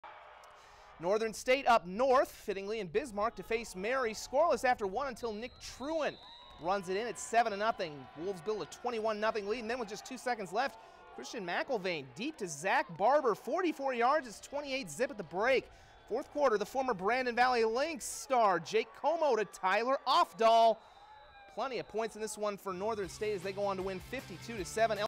Wolves Howl At Mary
Northern State wins at Mary 52-7.